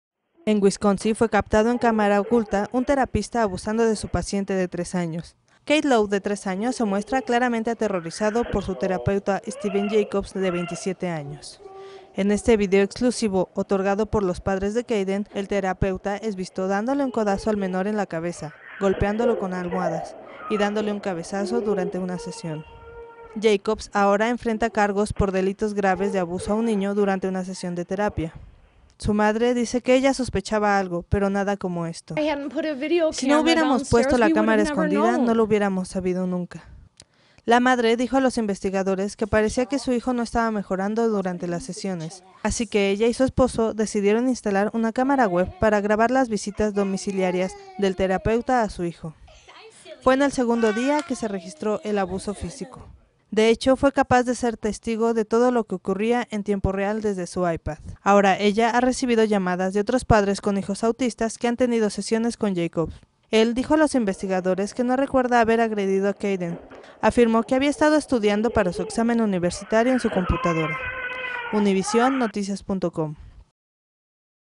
El abuso físico contra un niño autista fue captado en video con una cámara oculta -- Online
El terapeuta ahora enfrenta una demanda en su contra por maltratar a Caeden Lowe, un niño autista a quien debía cuidar.
El abuso físico contra un niño autista fue captado en video con una cámara oculta